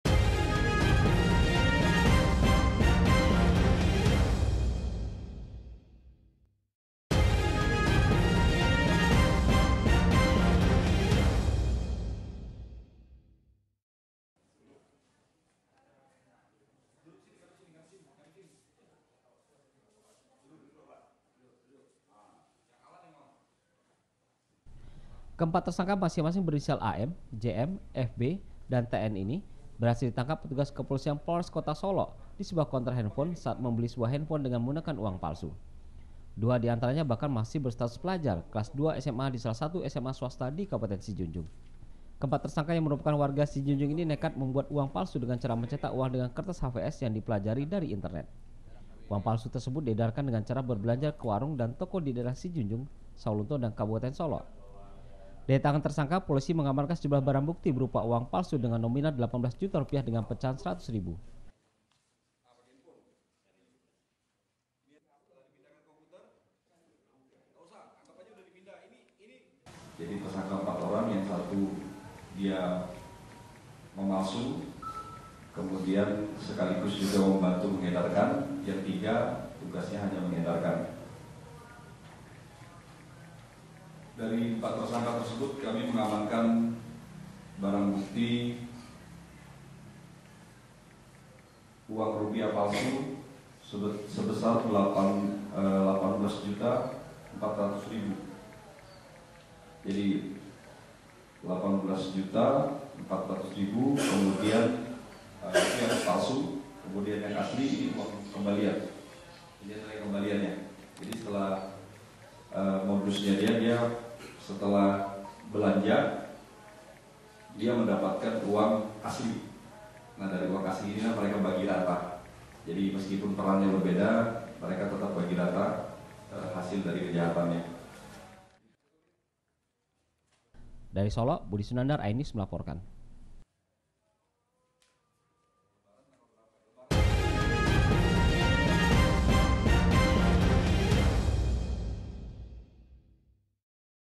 BUAT DAN EDARKAN UANG PALSU PELAJAR DIAMANKAN
Membuat dan mengedarkan uang palsu 4 orang remaja dibekuk petugas kepolisian resort solok kota, ironisnya, dua diantaranya masih berstatus pelajar sma.
 dari tersangka petugas mengamankan sejumlah uang palsu dengan nominal 18 juta rupiah, yang dicetak dengan komputer yang dipelajari dari internet.